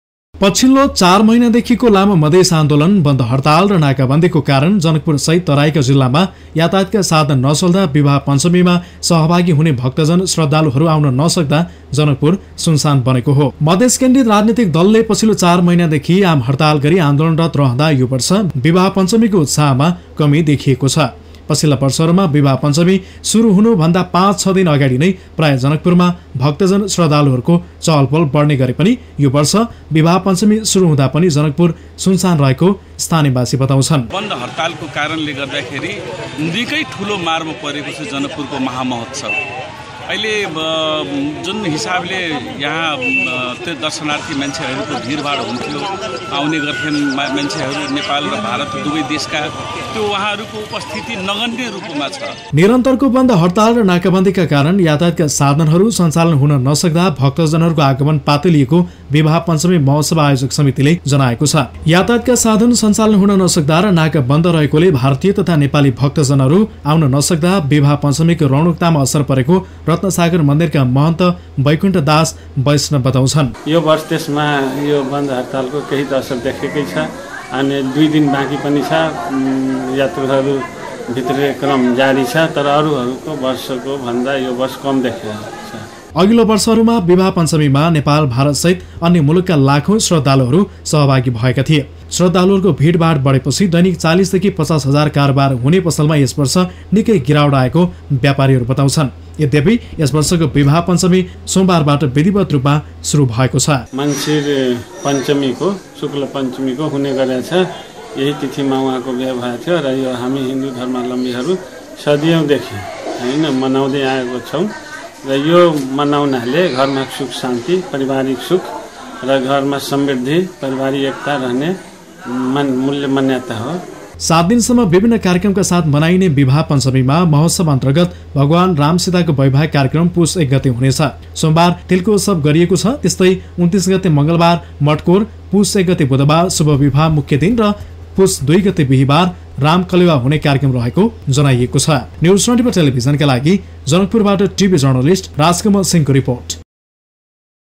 जनकपुरमा धूमधामका साथ मनाईने विवाह पञ्चमी महोत्सव सुरु - NEWS 24
जनकपुरमा धूमधामका साथ मनाईने विवाह पञ्चमी महोत्सव सुरु भएको छ । अघिल्लो वर्षमा विवाह पञ्चमीमा सहभागी हुने श्रद्धालुहरुको निकै भिडभाड हुने जनकपुर यसपटक भने जारी आन्दोलनका कारण सुनसान झै बनेको छ ।